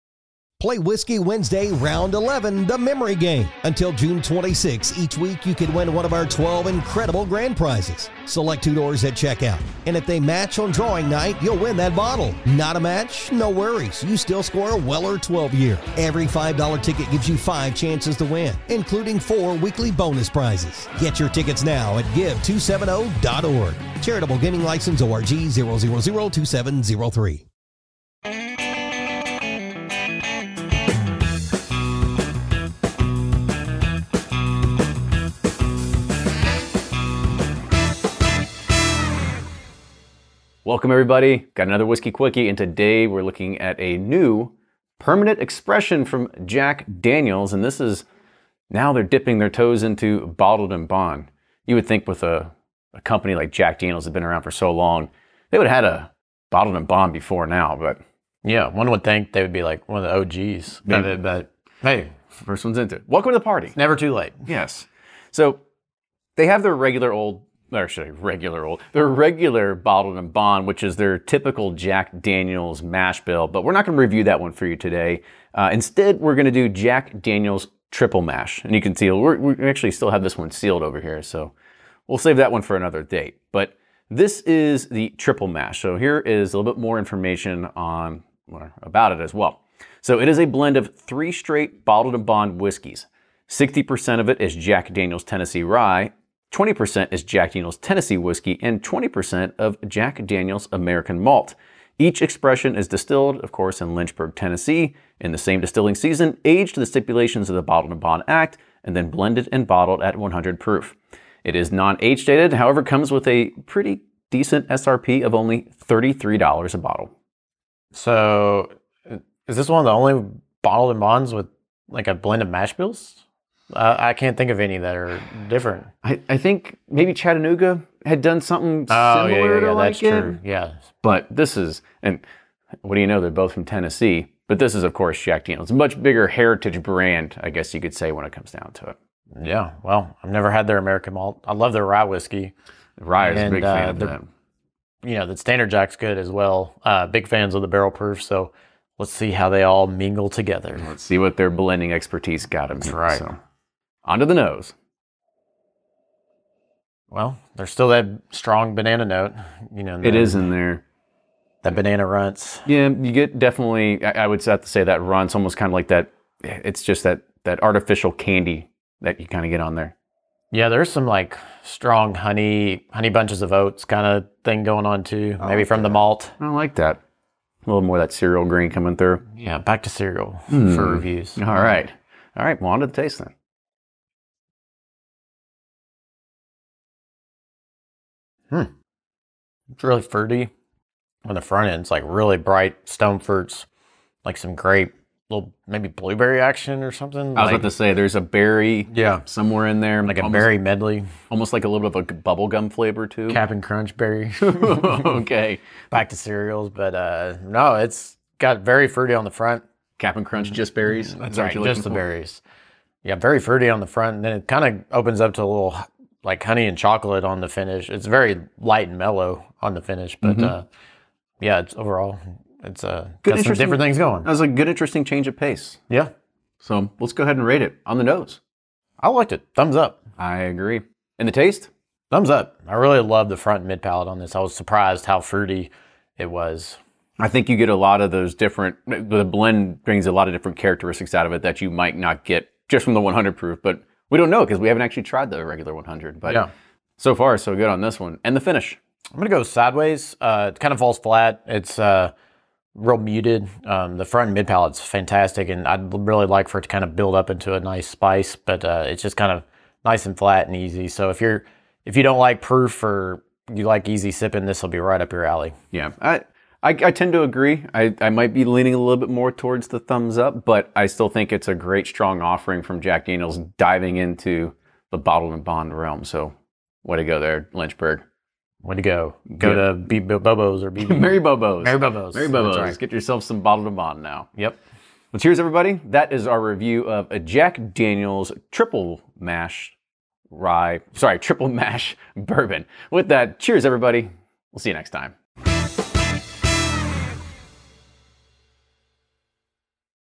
Whiskey Quickie: Jack Daniel’s Triple Mash Whiskey Review
On this Whiskey Quickie by Bourbon Pursuit, we review Jack Daniel’s Triple Mash. This non-age stated whiskey is 100 proof and $33 MSRP. Let us know what you think. Cheers!